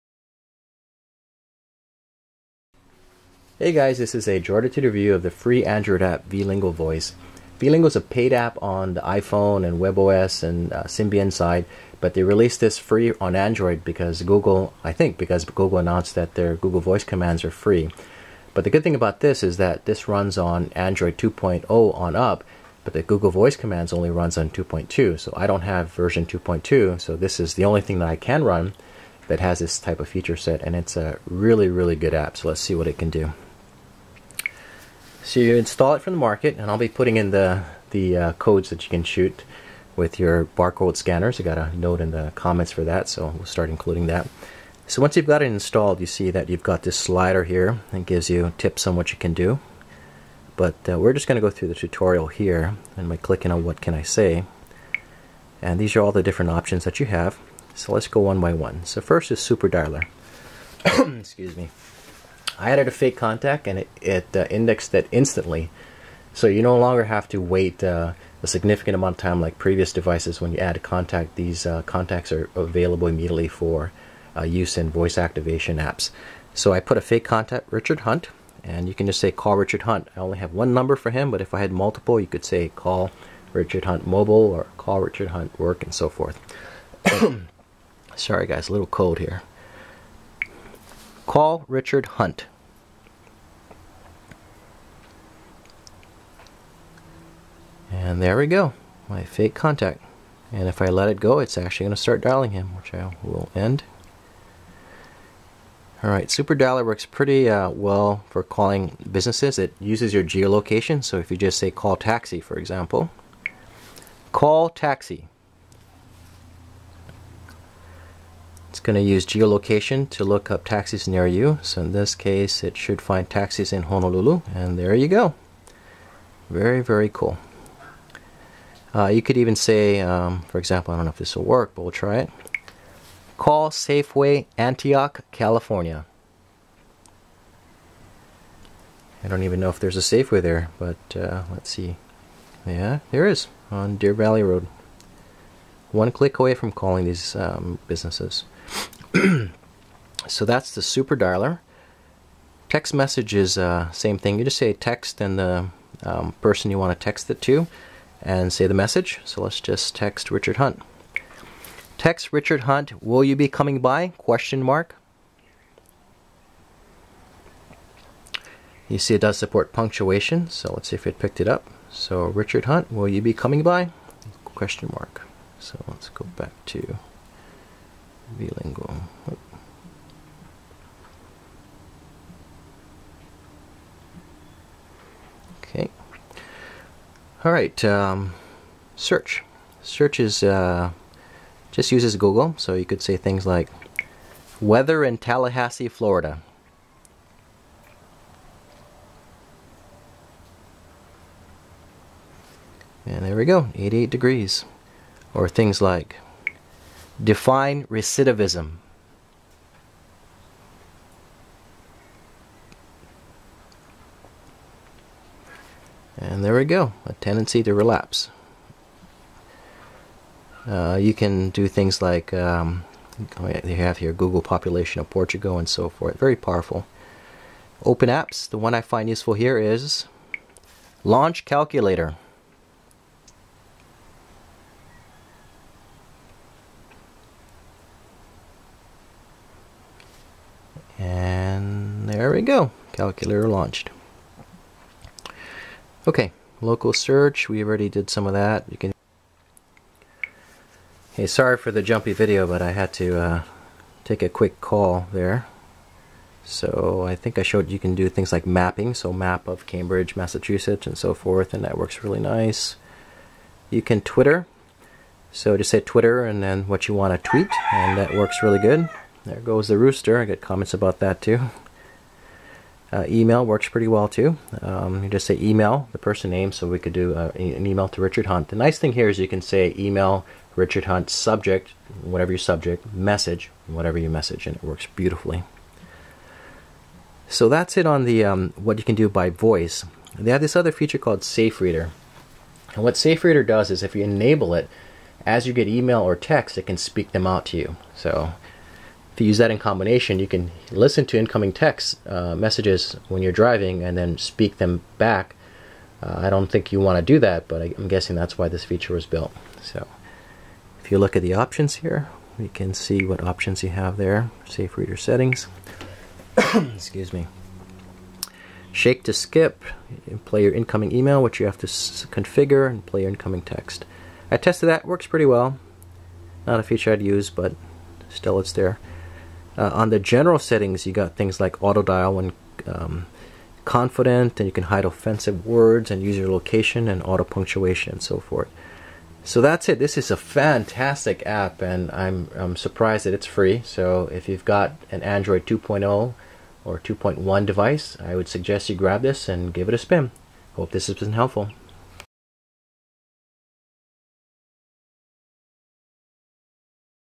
Control your Android device by voice: Review of Vlingo Voice
When I first saw a video introduction to Google's Voice Actions, I was blown away. Sadly, I can't run it, at least not until I upgrade my device to Android 2.2. I'm not alone. Most of us using Android are not yet using 2.2. But what if we want features like Voice Actions? Well if you are running at least Android 2.0 you can with a free app called Vlingo Voice Vlingo used to be a paid app but I think in response to Voice Actions being released for free, they followed suit with their Android version. They also offer Vlingo for the iPhone, Blackberry, and Nokia (some free, some paid).